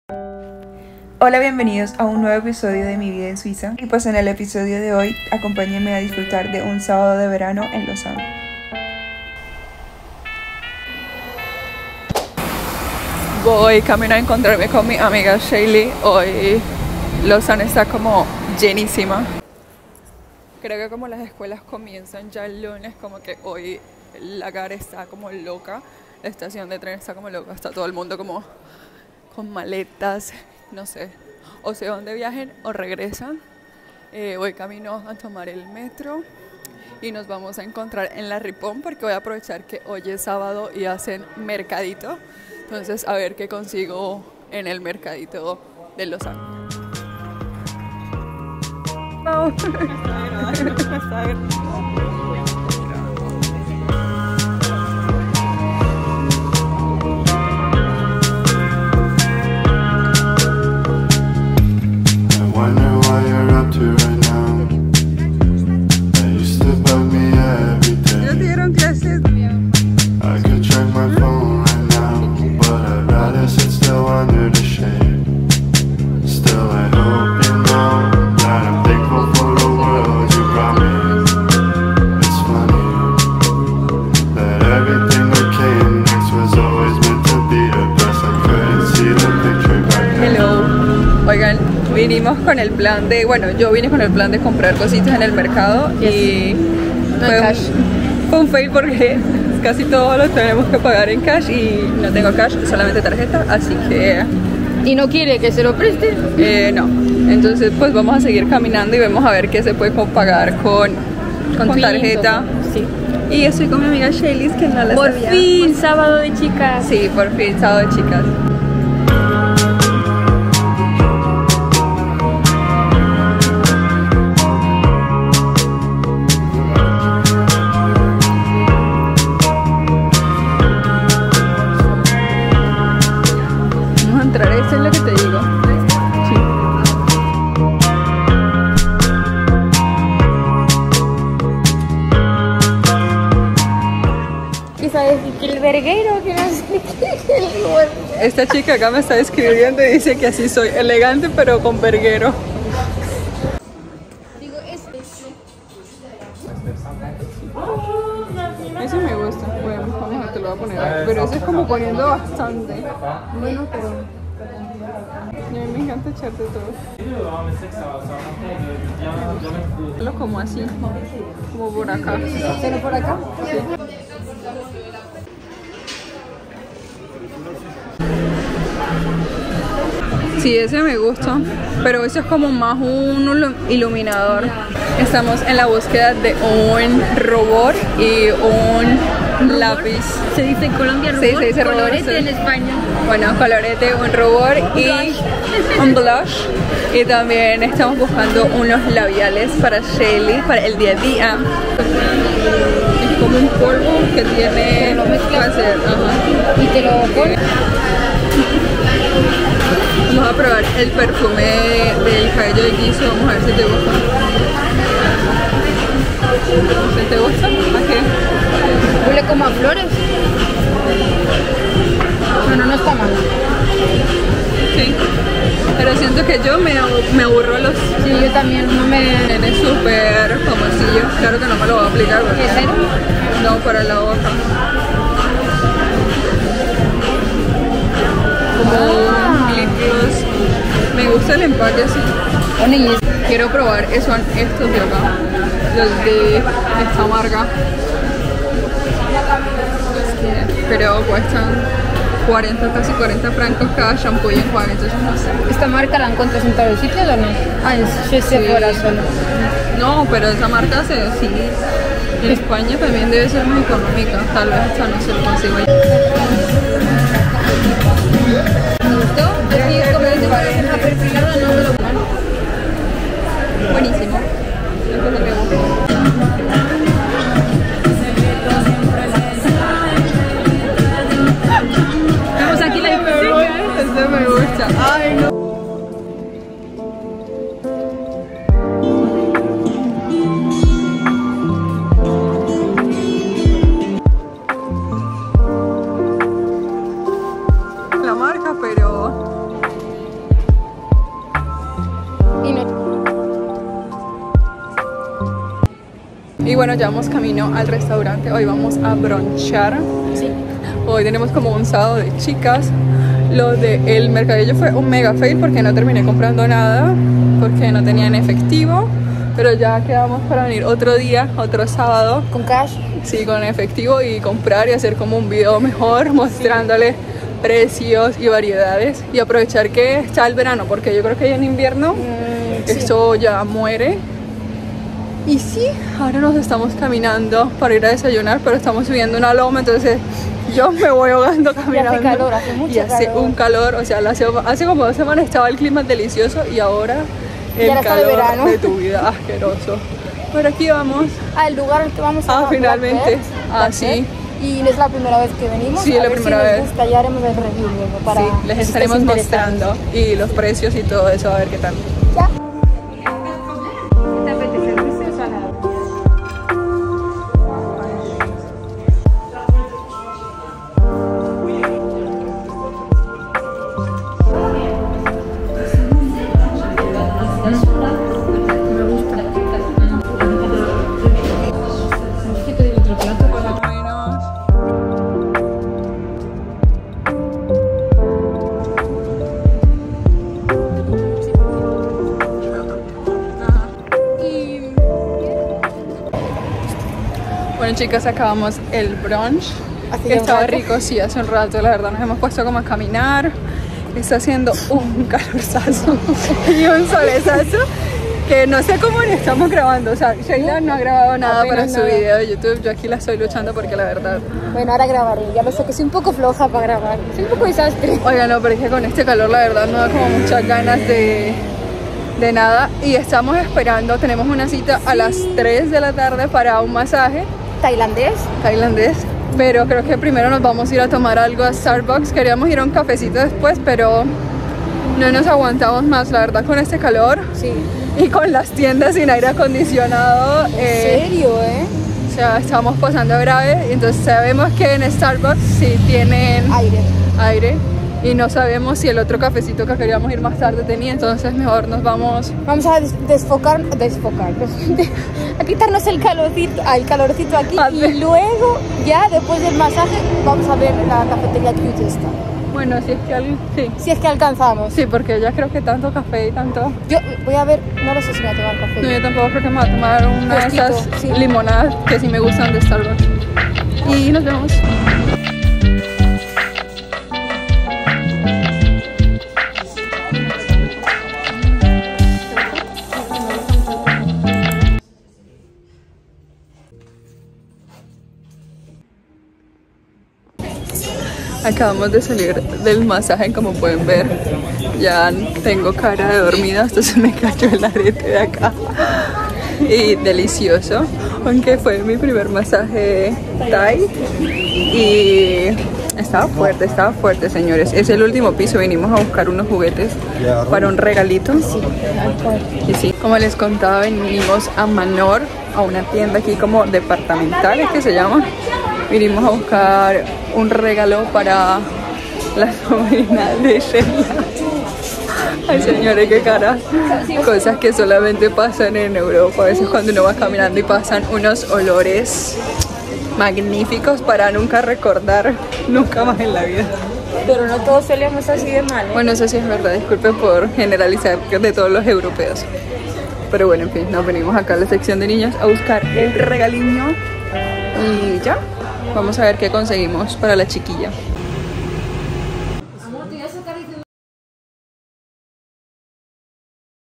Mi vida en Suiza| un sabado en Lausanne ,shopping,brunch y mercadito suizo🇨🇭
en suiza